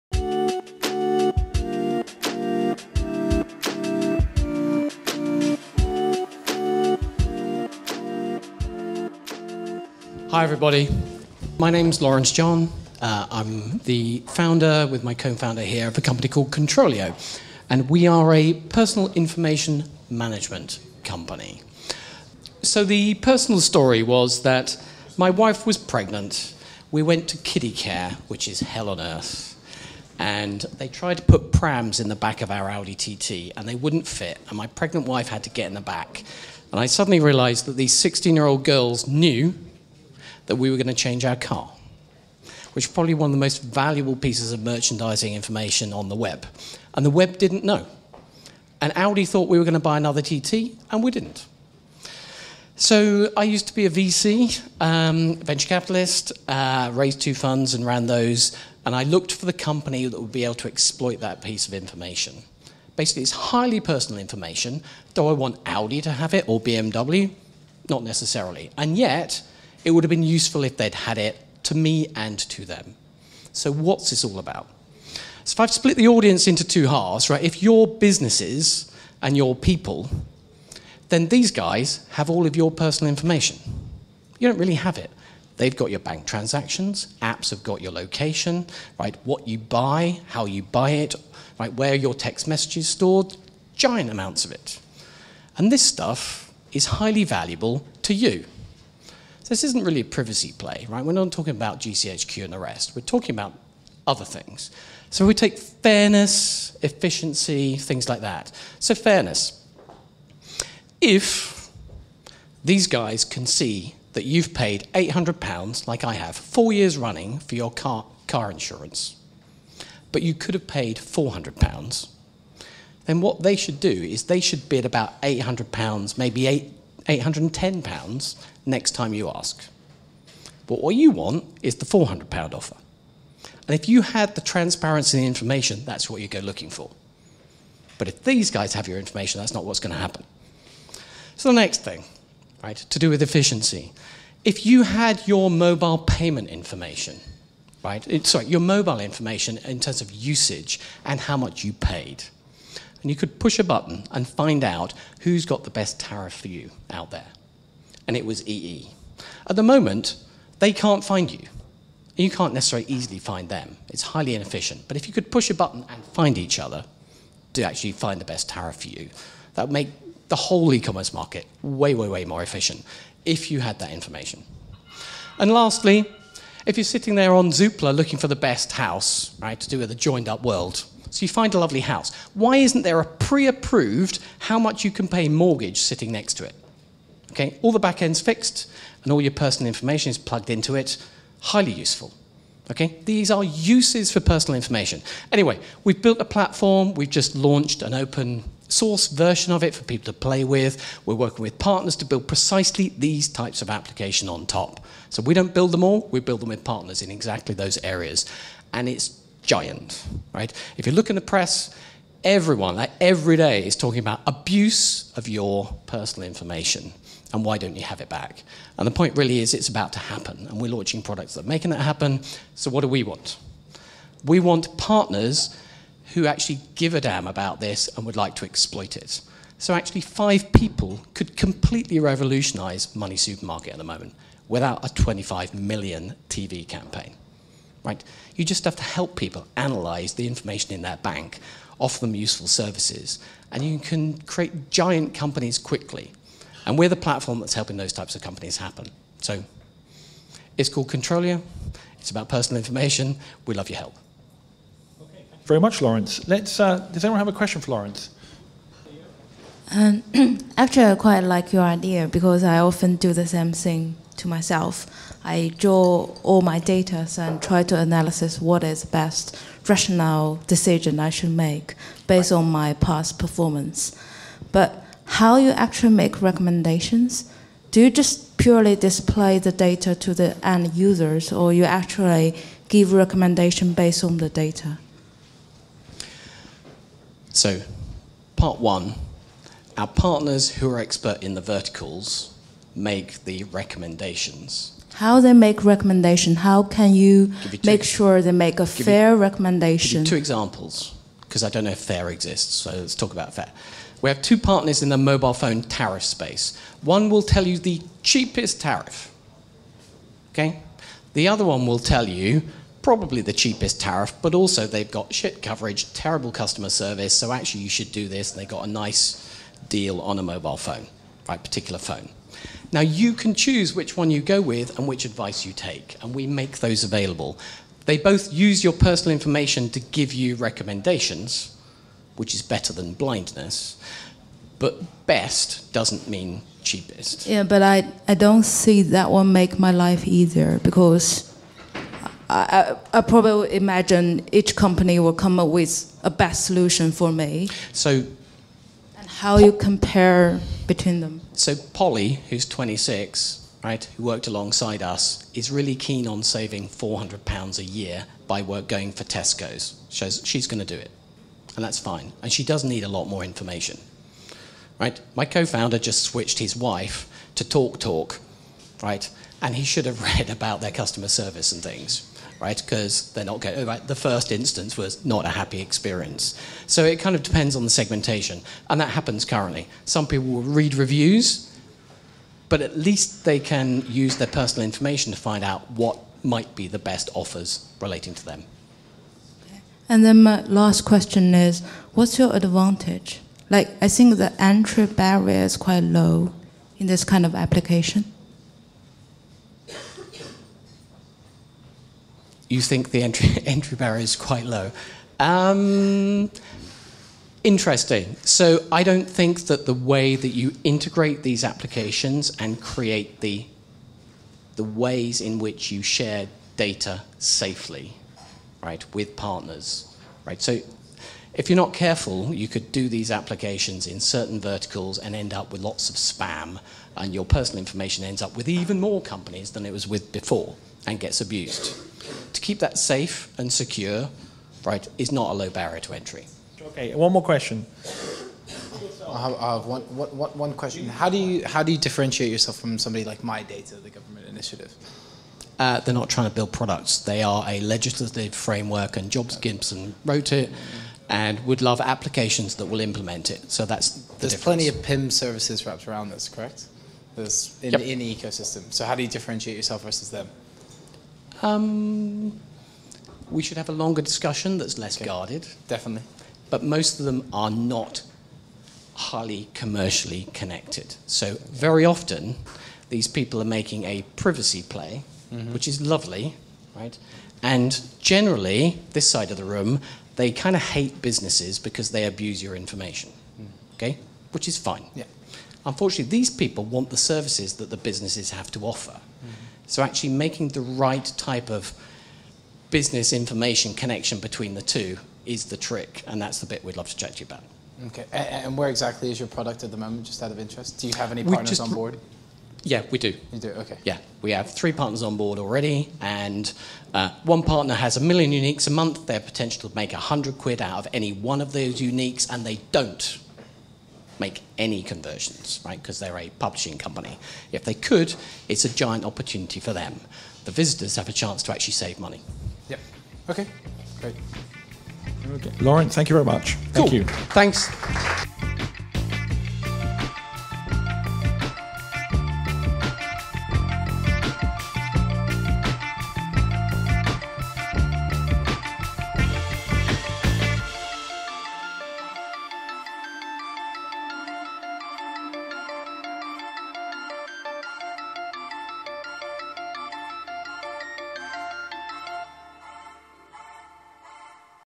Laurence John, CTRLio
Laurence John, Founder of CTRLio, who's mission is to empower people with their personal information. This will help to create a connected world that is fairer, more efficient and joined up

Speaking at our FinTech Open Mic Night on the 4th November at The Pause Bar in London